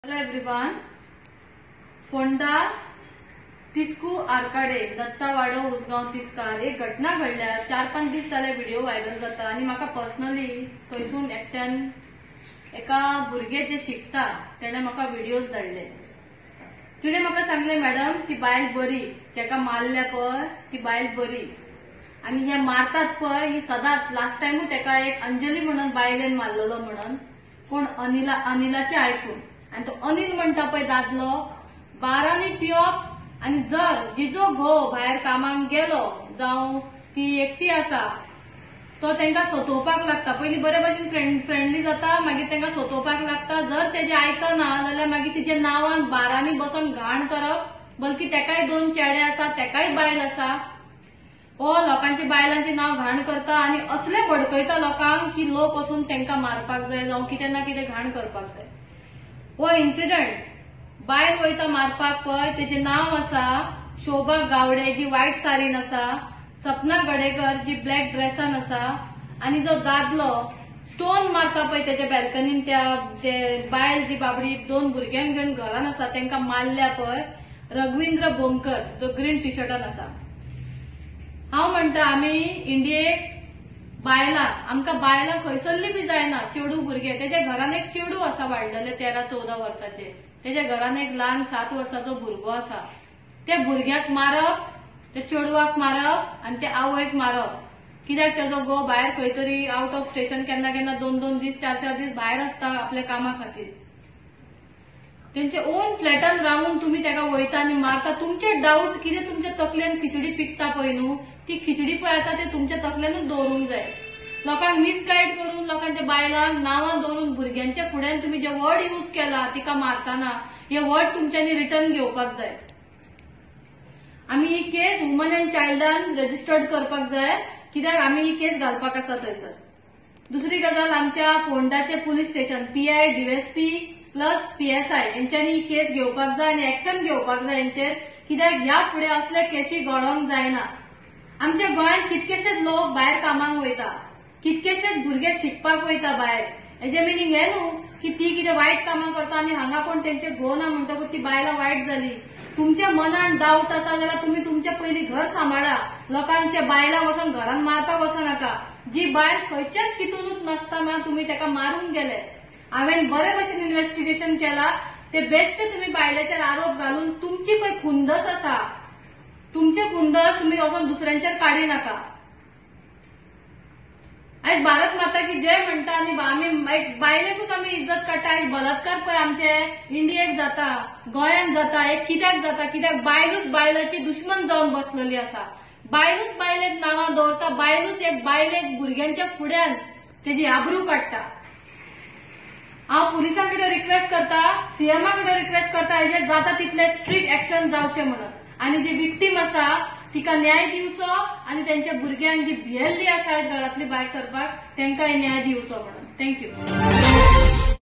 The mother and her children have been beaten up by neighbors at Usgaon Tisk,Ponda- Goa
The mother and her children have been beaten up by neighbors @ Tisku Arcade building, Datta Waddo, Usgaon tiskar, Ponda- Goa
Is women's empowerment really exist in this cruel world?

The mother and her children have been threatened by neighbors in the absence of her husband.
Now children are also not safe in front of these morons.
Does a woman have respect for another woman? A BIG QUESTION MARK???
Respect for the women has been taken up for a toss by women itself.

The victims should serve justice. - Shreha Dhargalkar.